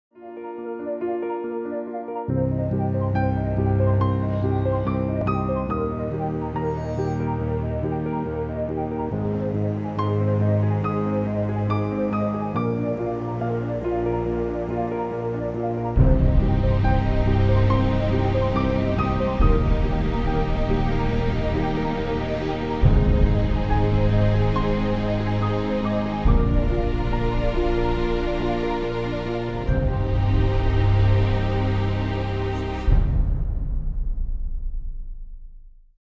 3D Product Animation - TESY Conveco Cloud Video
The 3D product animation was created for the latest TESY's panel electric convector. Conveco Cloud are among the latest additions to the TESY Cloud family with control via Internet.